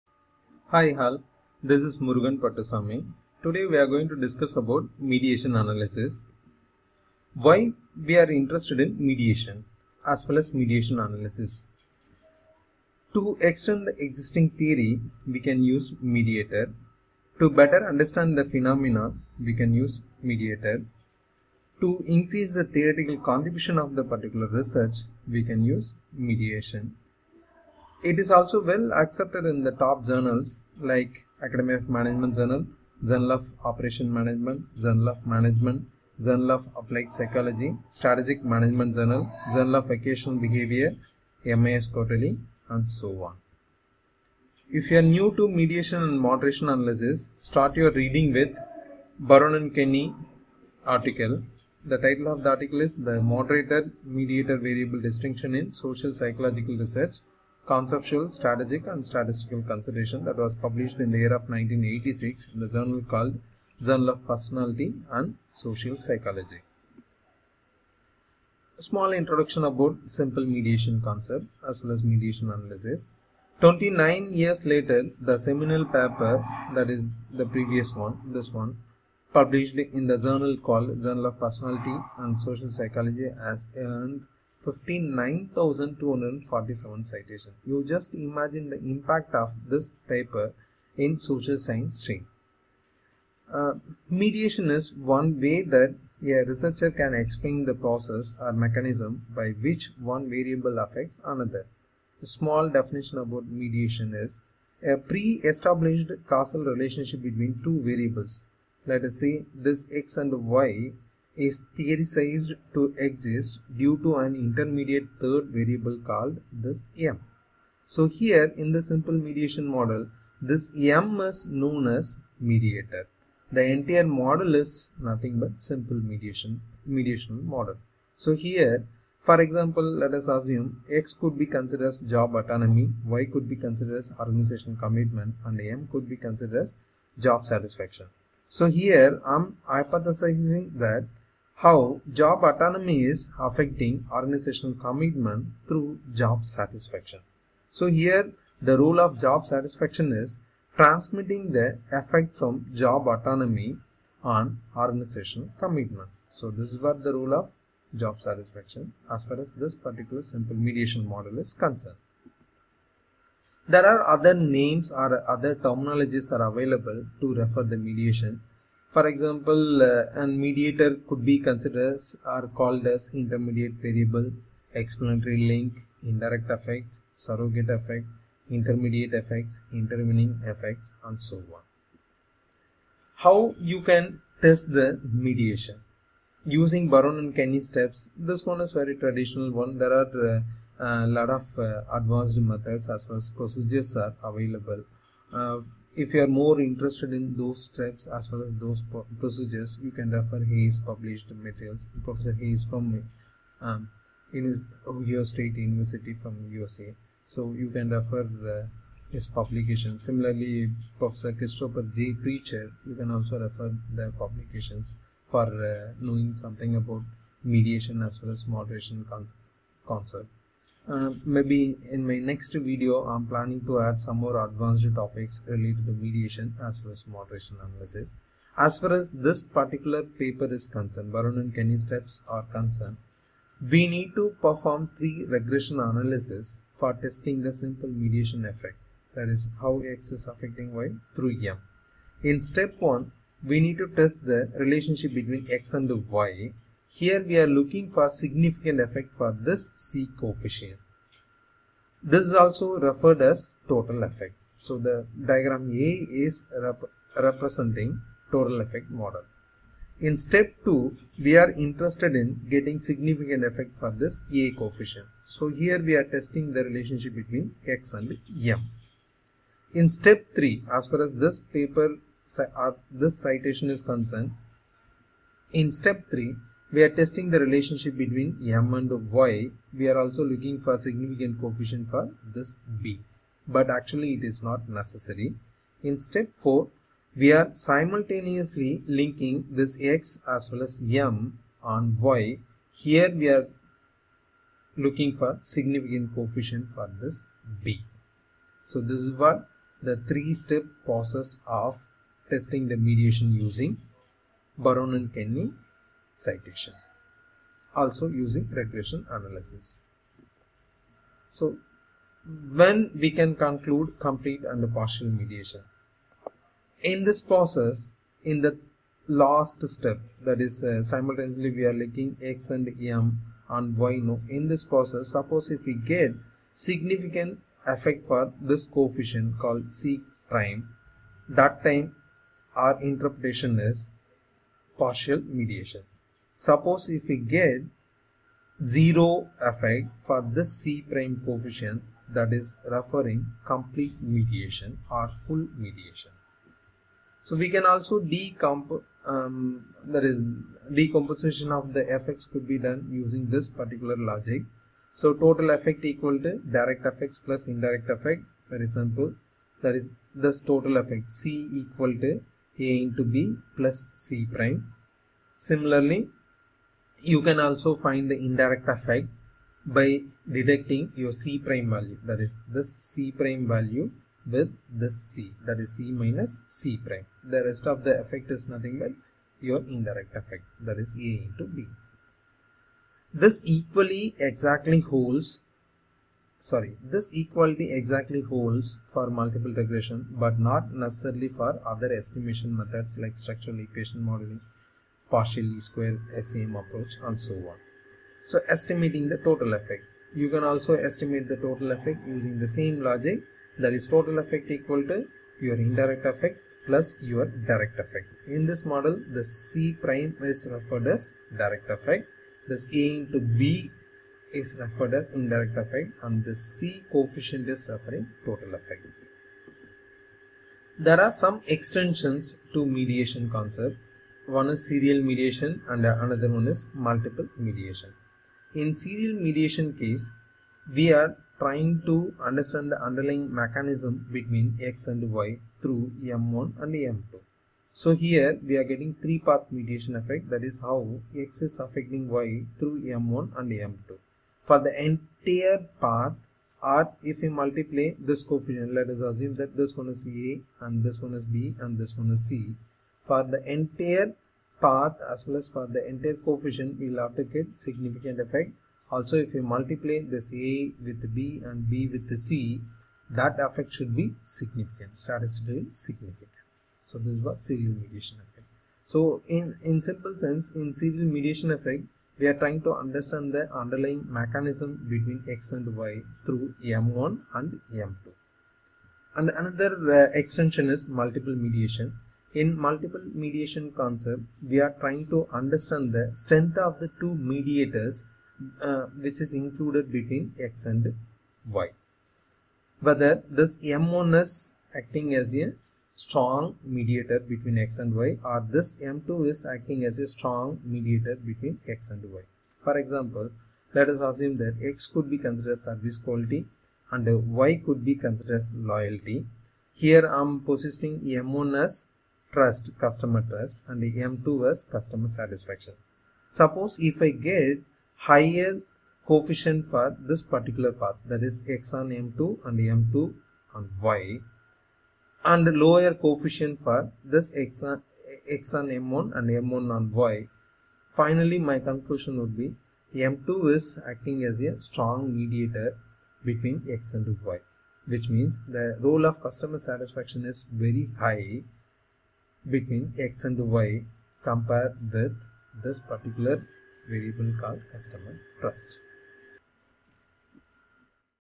Mediation analysis
This video is about explaining the concept of simple mediation analysis and testing using regression analysis based on Baron and Kenny (1986) citation.